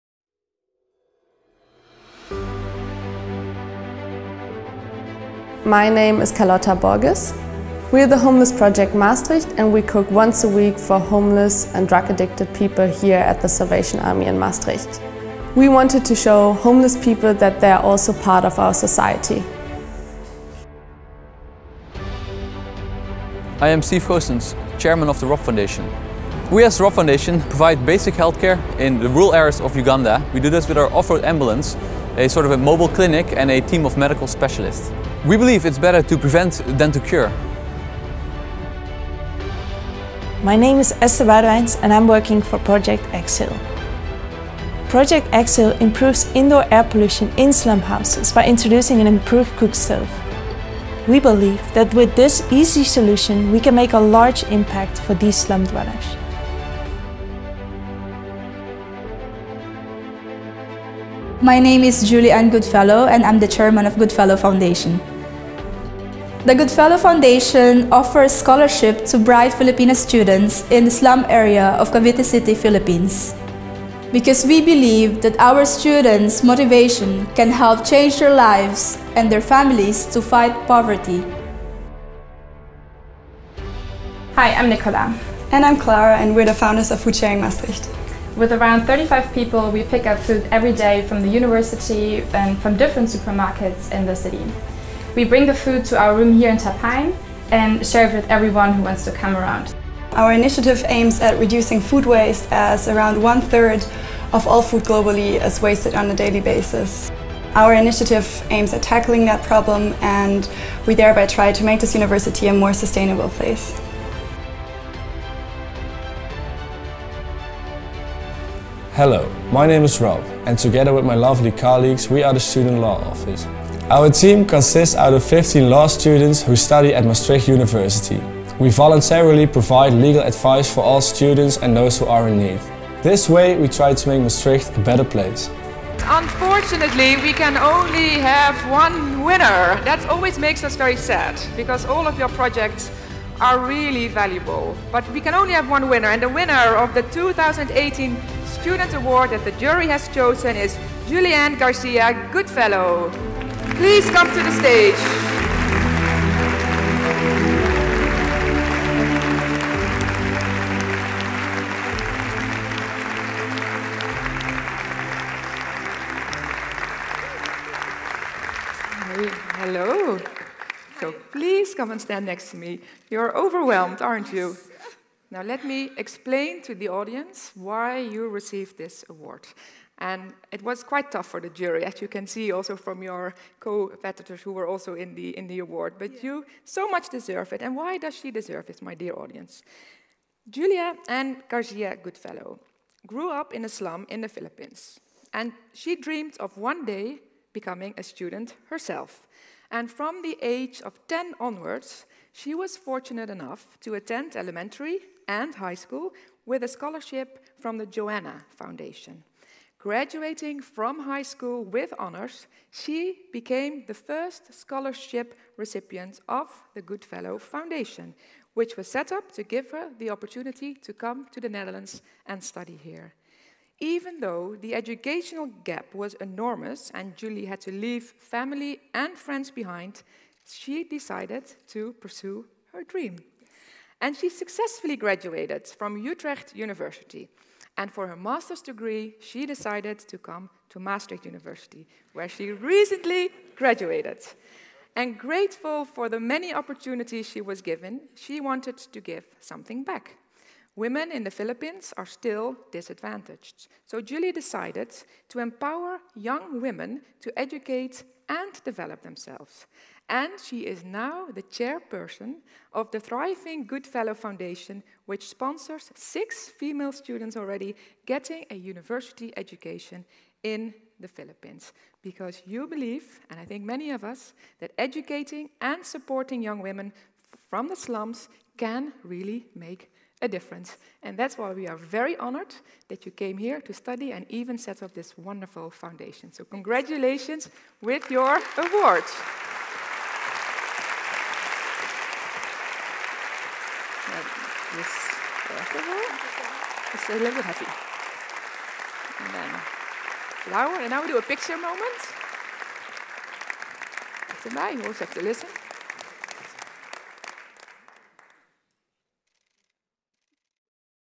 UM Student Award nominations and winner of OAY 2018/19
The six nominations for the 2018 UM Student Award are Carlotta Borges of the Homeless Project Maastricht, Steve Goossens of the Rob Foundation, providing basic healthcare in rural areas in Uganda, Esther Boudewijns of Project Exhale, set up to improve indoor air pollution in slum houses, Julie Ann Garcia Goodfellow who has given her name to the Goodfellow Foundation to empower women in her native Philippines through education, Nicola Bauer and Klara Strecker, who try to make the UM more sustainable through food sharing, and Rob Tonen, providing legal advice to students and others in Maastricht.